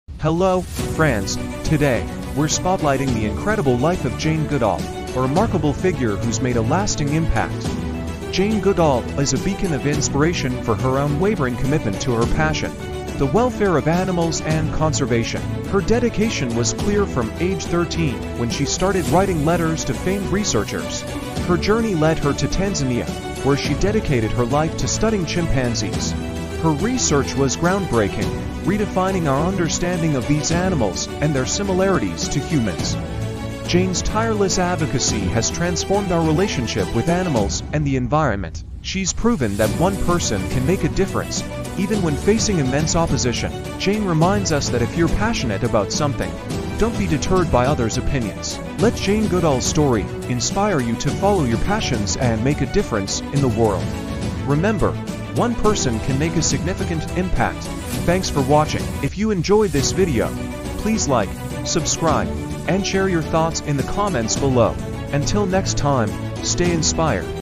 The Story of Jane Goodall
Jane Goodall, primatologist and conservationist, dedicated her life to studying chimpanzees and advocating for animal welfare. Her groundbreaking research and tireless advocacy have made a significant impact on our understanding of animals and the environment.